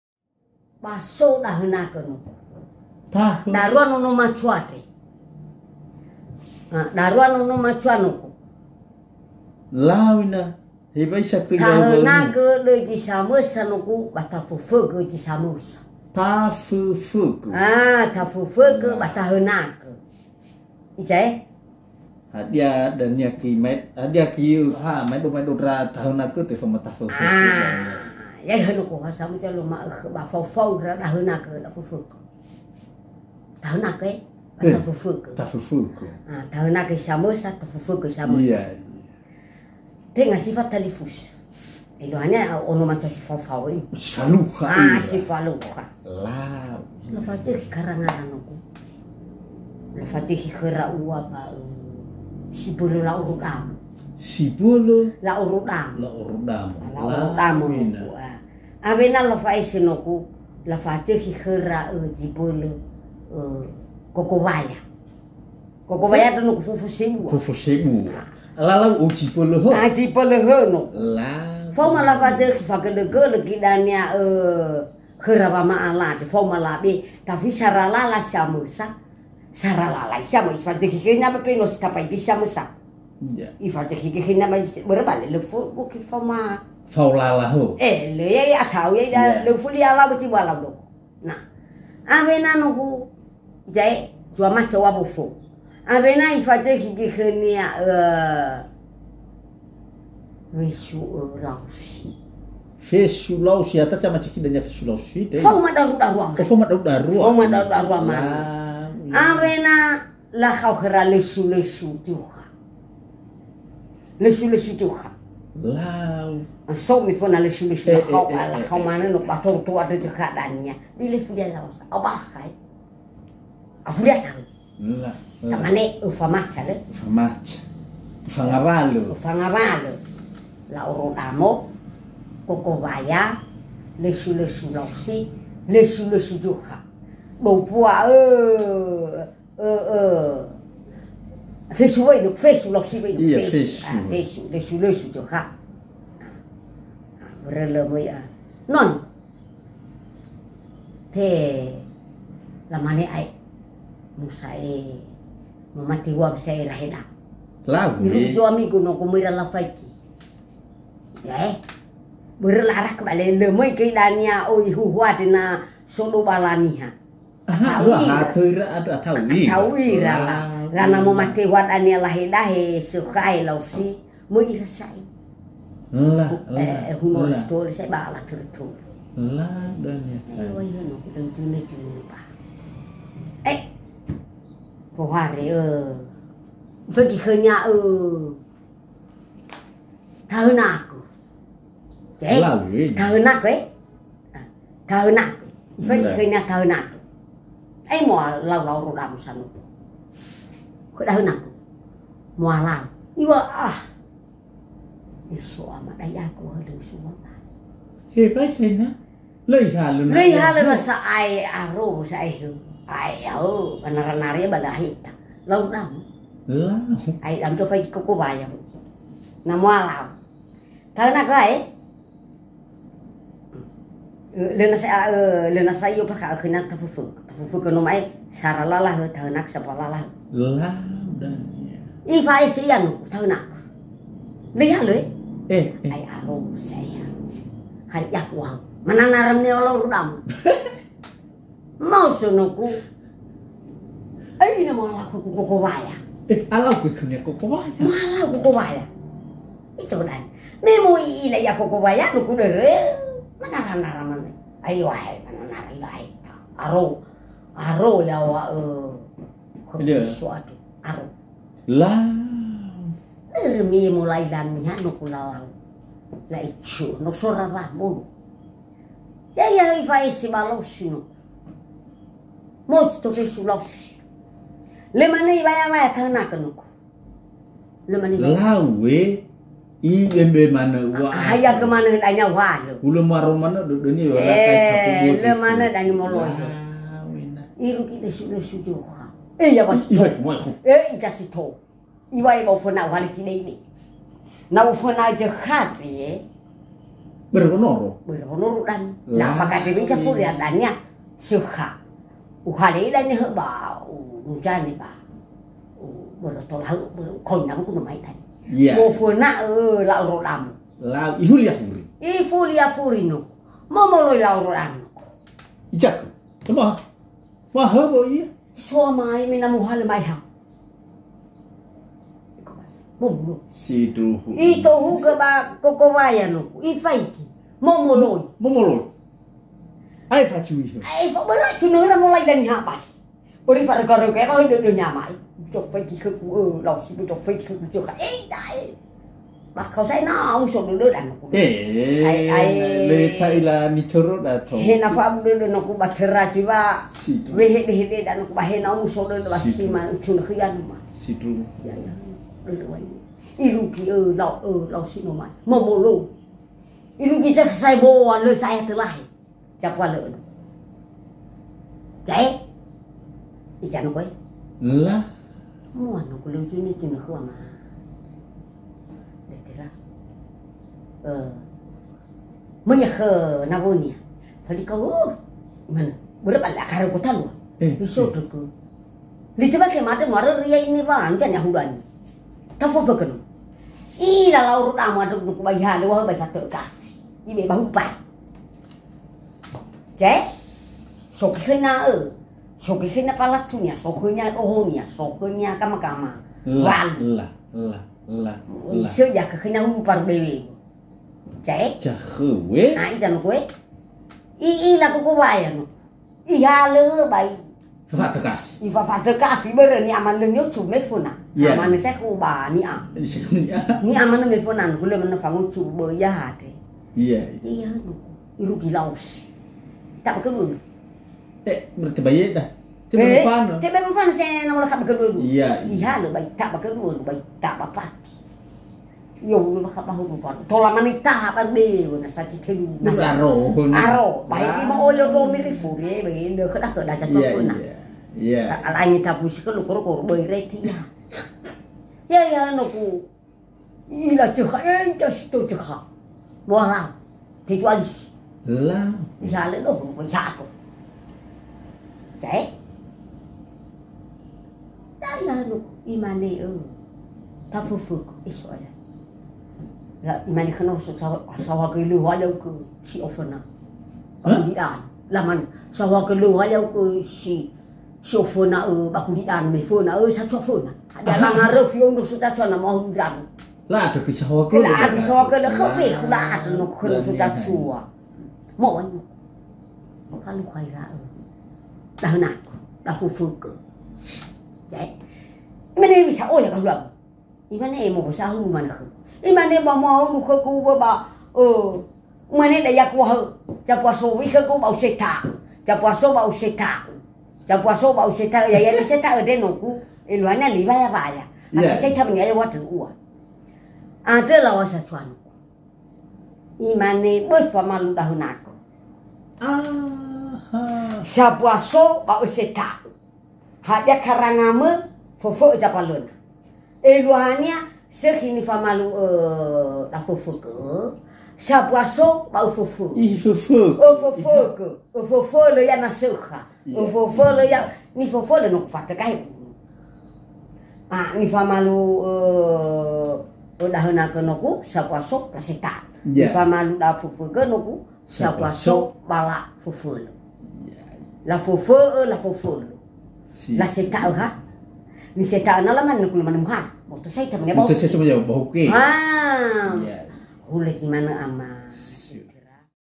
CERITA RAKYAT NIAS ‖ Maedo maedo ‖ MALU DAFÖFÖ'Ö-TAHÖNAGÖ
Cerita rakyat ini di Nias disebut Maedo-maedo, manö-manö bahkan ada yang mengatakan amaedola tradisi ini merupakan salah satu kearifan lokal yang tergolong dalam warisan budaya tak benda.
Cerita ini dua pemuda yang berburu di hutan mereka adalah Taföfö'ö dan Tahönagö, bagaimana ceritanya silahkan ikuti ceritanya.